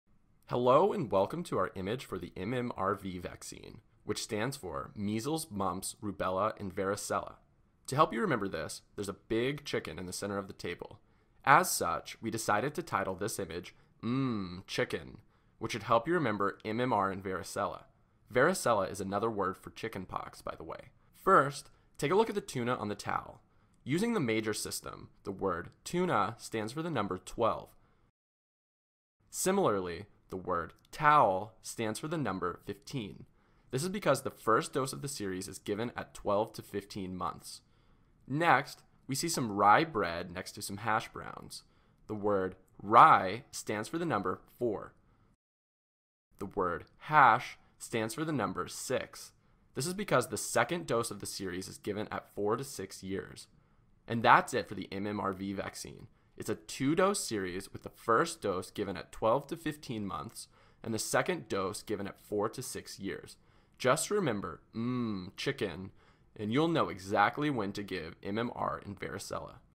MMRV Immunization Schedule - Memory Palace - "Mmm, Chicken"
Learn the Immunization schedule for the MMRV vaccine. Based on CDC guidelines. Perfect for pediatrics medical students, interns, and more.